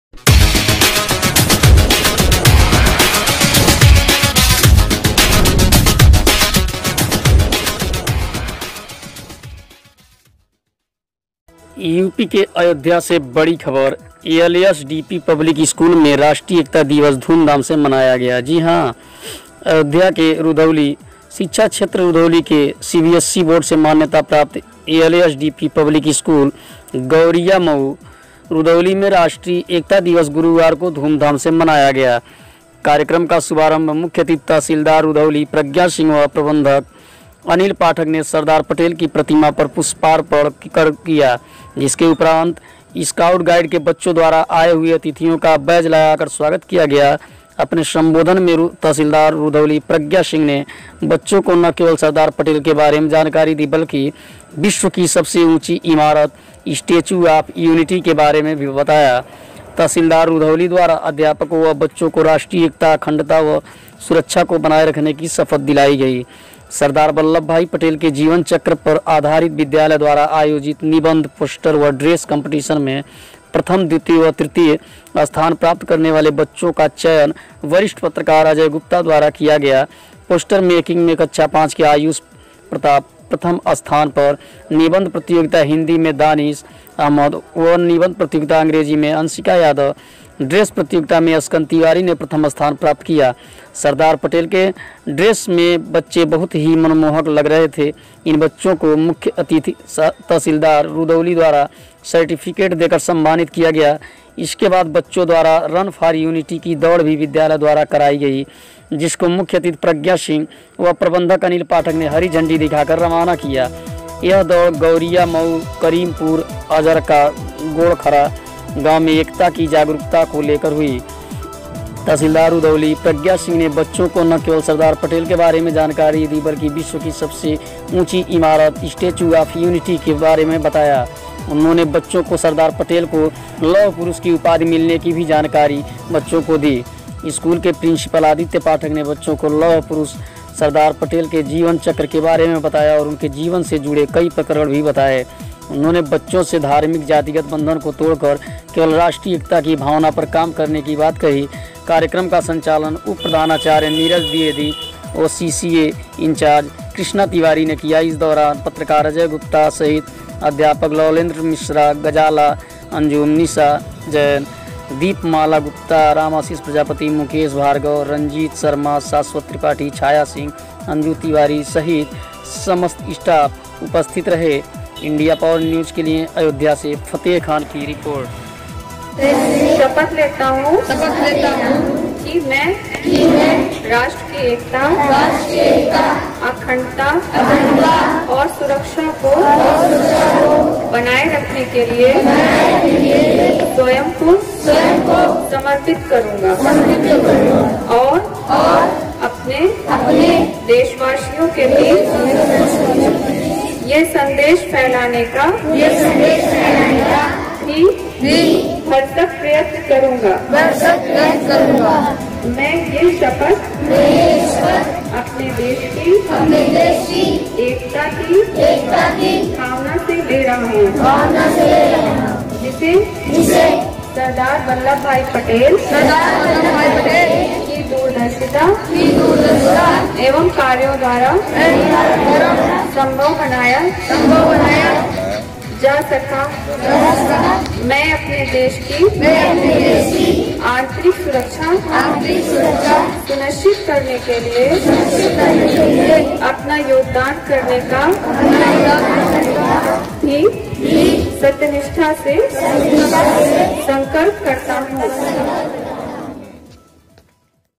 अयोध्या में धूमधाम से मनाया राष्ट्रीय एकता दिवस,प्रज्ञा सिंह ने सरदार पटेल के जीवनी पर डाला प्रकाश
अयोध्या में एलएसडीपी स्कूल में मनाया गया राष्ट्रीय एकता दिवस इस अवसर पर तहसीलदार प्रज्ञा सिंह ने सरदार बल्लभ भाई पटेल के जीवनी पर प्रकाश डालते हुए सभी बच्चों को शपथ दिलाई।